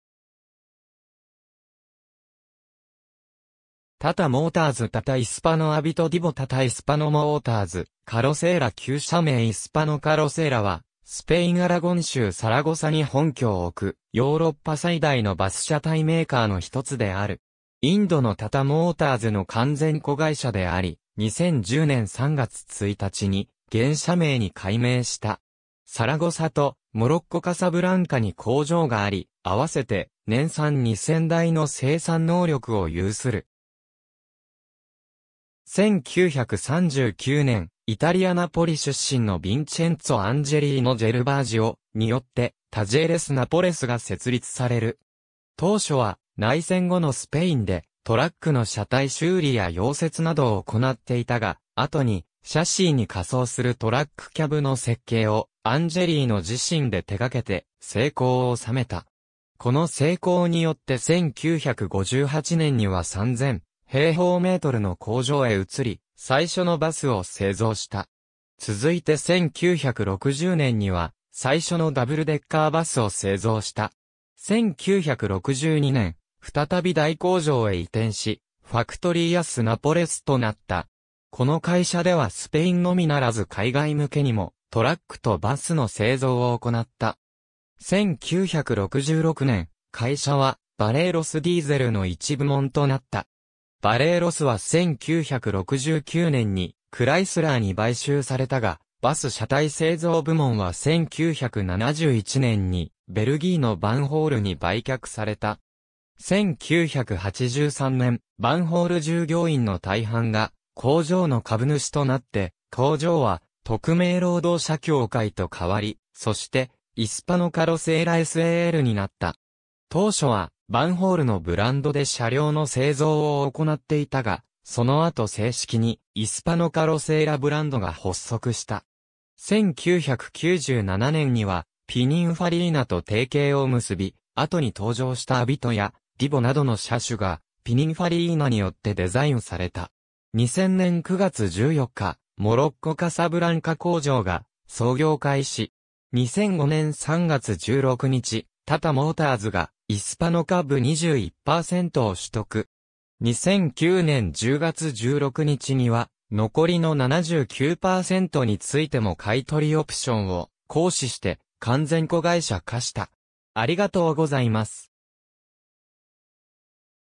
タタ・イスパノ
スペインの自動車メーカー・ブランド
タタ・モーターズ
タタ・モーターズ 」 タタ・イスパノ アビト（MAN NG313-F） ディボ（メルセデス・ベンツのシャシー上に架装） タタ・イスパノ・モーターズ・カロセーラ (Tata Hispano Motors Carrocera S.A.) 、旧社名イスパノ・カロセーラ (Hispano Carrocera S.A.) は、スペイン・アラゴン州サラゴサに本拠を置く、ヨーロッパ最大のバス車体メーカーの一つである。
インドのタタ・モーターズの完全子会社であり、2010年3月1日に現社名に改名した。
サラゴサとモロッコ・カサブランカに工場があり、合わせて年産2,000台の生産能力を有する。
1939年、イタリア・ナポリ出身のヴィンチェンツォ・アンジェリーノ・ジェルヴァージオ (Vincenzo Angelino Gervasio) によってタジェーレス・ナポレス (Talleres Nápoles) が設立される。
当初は内戦後のスペインでトラックの車体修理や溶接などを行なっていたが、後にシャシーに架装するトラックキャブの設計をアンジェリーノ自身で手がけて成功を収めた。
この成功によって1958年には3000㎡の工場へ移り、最初のバスを製造した。
続いて1960年には最初のダブルデッカーバスを製造した。
1962年、再び大工場へ移転し、ファクトリーアス・ナポレス (Factorías Nápoles, S.A.) となった。
この会社ではスペインのみならず海外向けにもトラックとバスの製造を行った。
1966年、会社はバレイロス・ディーゼル (Barreiros Diesel, S.A.) の一部門となった。
バレイロスは1969年にクライスラーに買収されたが、バス車体製造部門は1971年にベルギーのバンホールに売却された。
1983年、バンホール従業員の大半が工場の株主となって、工場は匿名労働者協会 (Sociedad Anónima Laboral) と変わり、そしてイスパノ・カロセーラS.A.Lになった。
当初はバンホールのブランドで車両の製造を行っていたが、その後正式にイスパノ・カロセーラブランドが発足した。
1997年にはピニンファリーナと提携を結び、後に登場したアビトやディボなどの車種がピニンファリーナによってデザインされた。
2000年9月14日、モロッコ・カサブランカ工場が操業開始。
2005年3月16日、タタ・モーターズがイスパノ株21%を取得。
2009年10月16日には残りの79%についても買取オプションを行使して完全子会社化した。